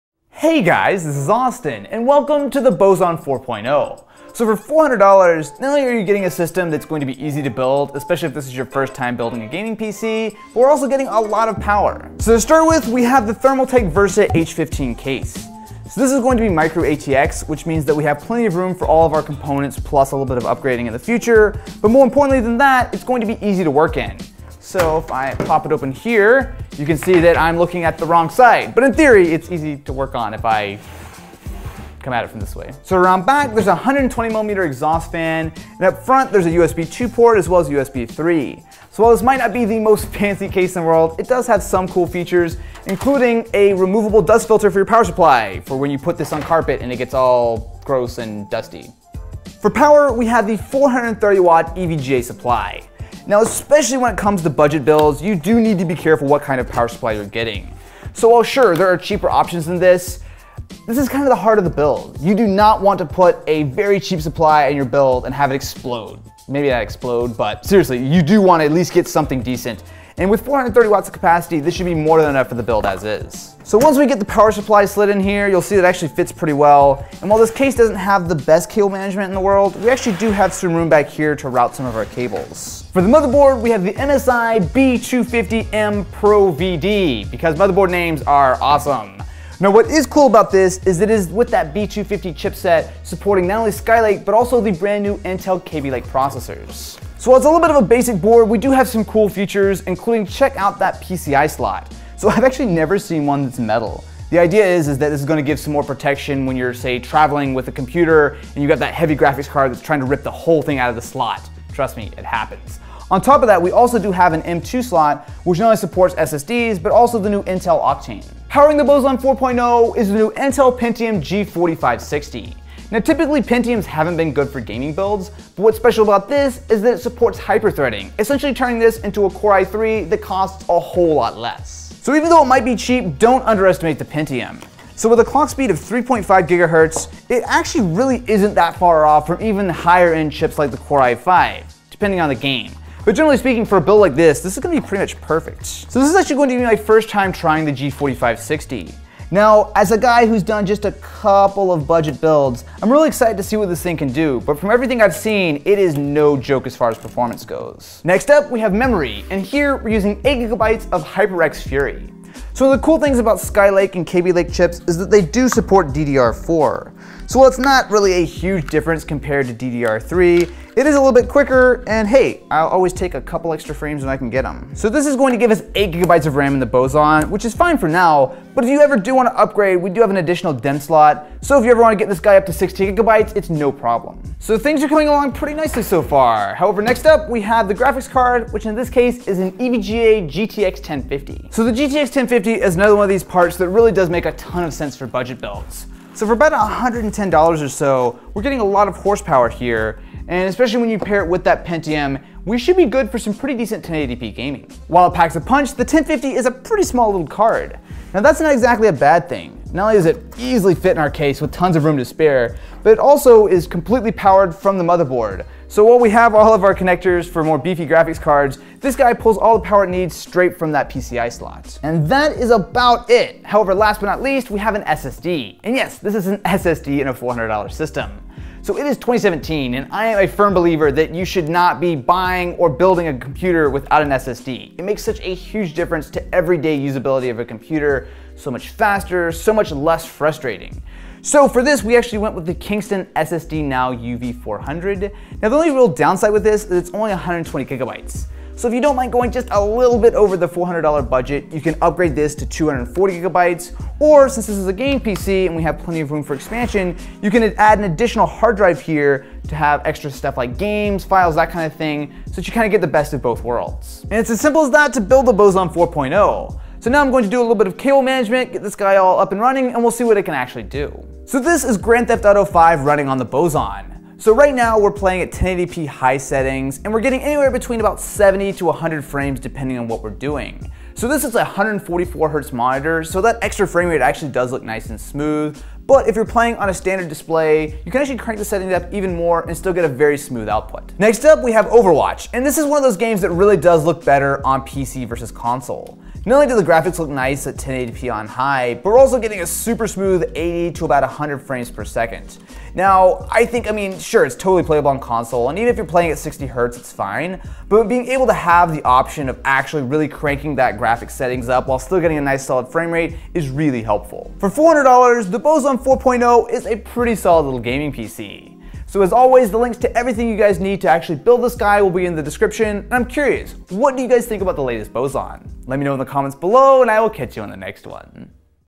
Boson 4.0 Review - $400 Gaming PC Build on 2017
For $400 the Boson 4.0 is a fresh gaming PC build for 2017!
Intel Pentium G4560 Buy
EVGA GTX 1050 Buy
MSI B250M PRO-VD Buy
8GB Kingston HyperX Fury Buy
120GB Kingston UV400 Buy
430W EVGA PSU Buy
Thermaltake Versa H15 Buy

We are here everyday to review and rate all new about Best Electronics online :)